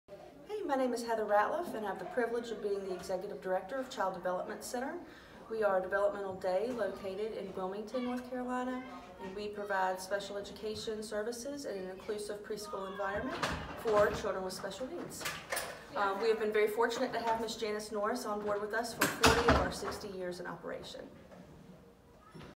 Child Development Center in Wilmington
The Child Development Center is a Wilmington preschool that serves children with special needs